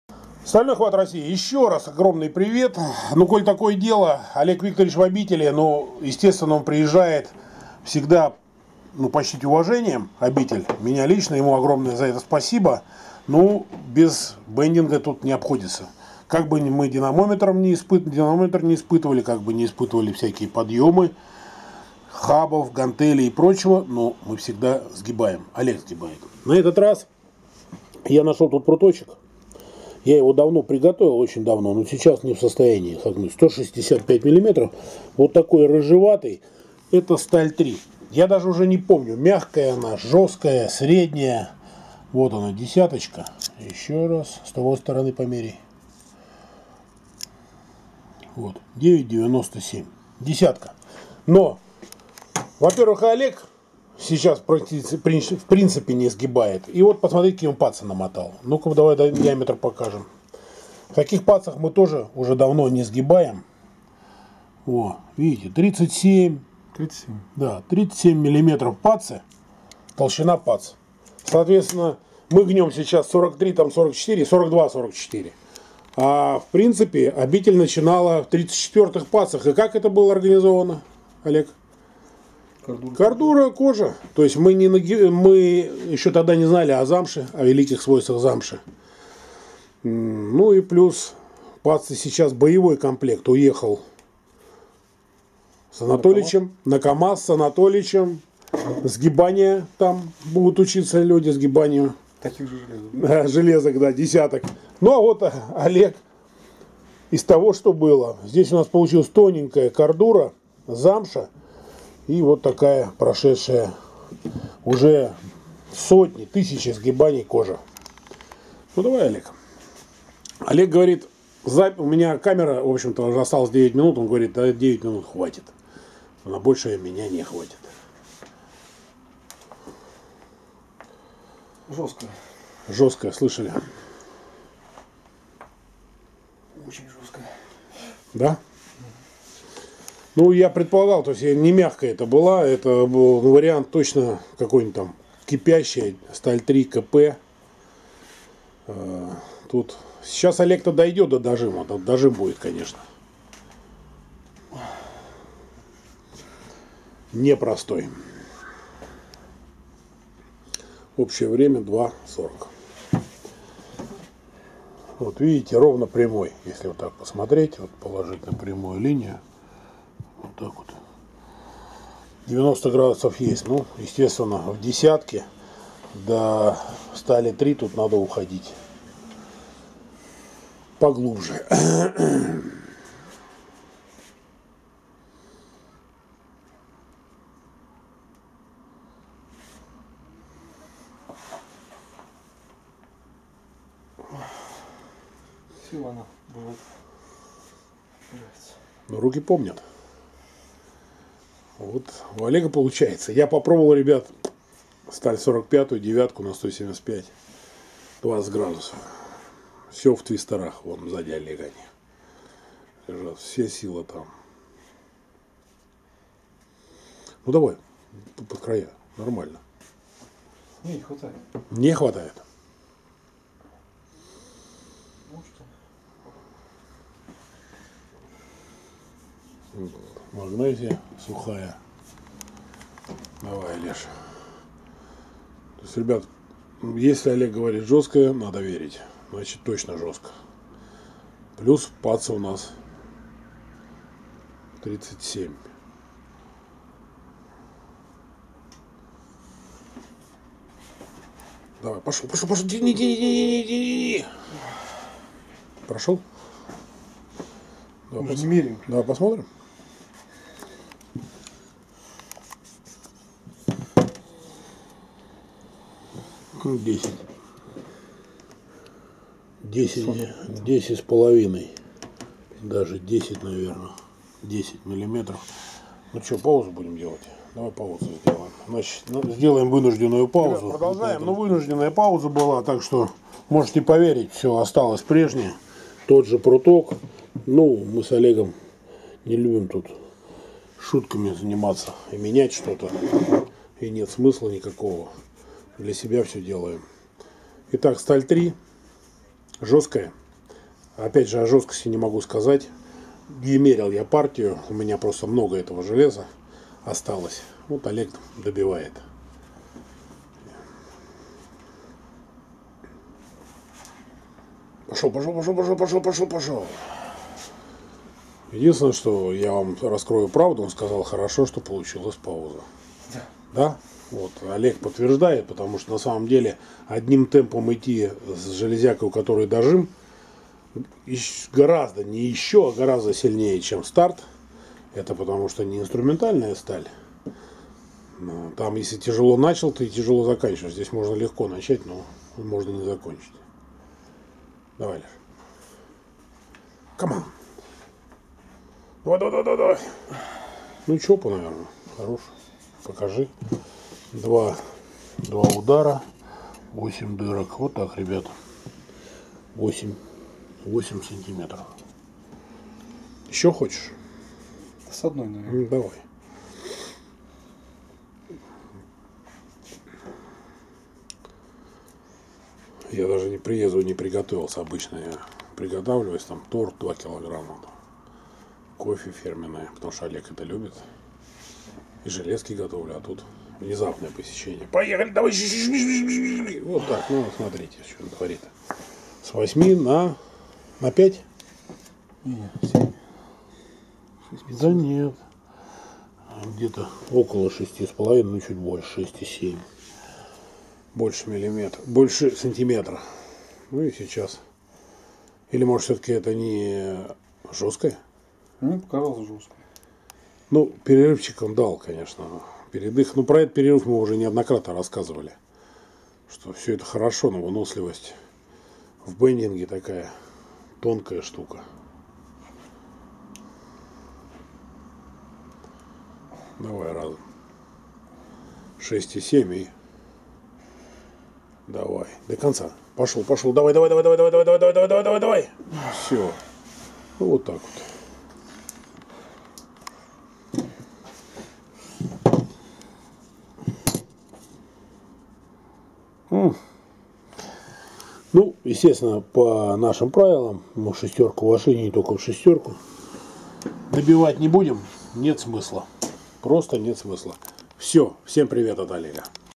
Олег сгибает ст.3кп в длине 165мм и диаметром 10мм
На данный момент Олег бэндинг практикует всё реже и реже,но сломать десятку из ст.3 для него не представляет сложности.